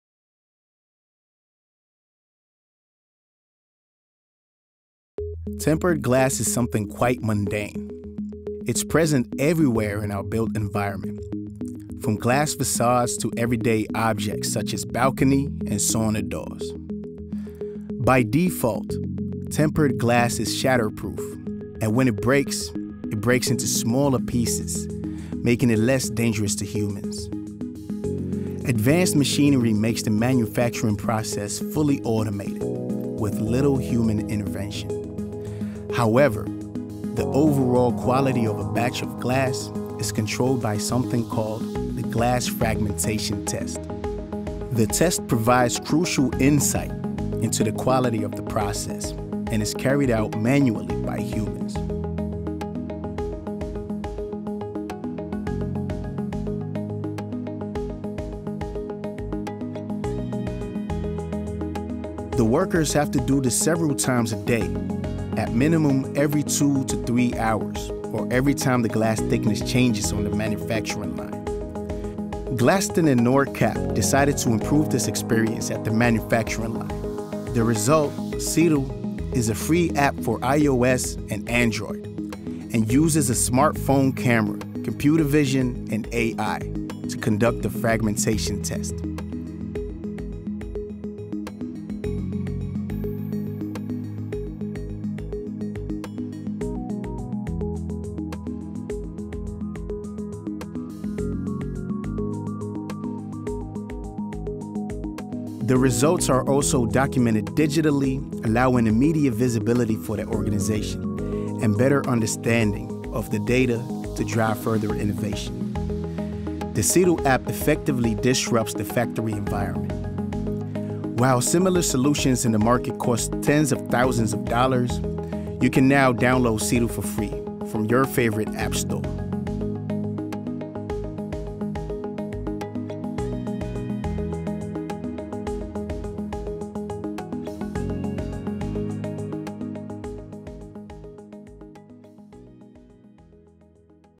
Glaston Siru App Update
Glaston Siru App is updated - now available in ANSI version as well. Siru App is a utility application for the mandatory glass fragmentation test in the glass tempering industry. The application combines artificial intelligence (AI) and augmented reality (AR) to analyze glass fragmentation patterns in an image. Both European (EN) and American (ANSI) standards are supported.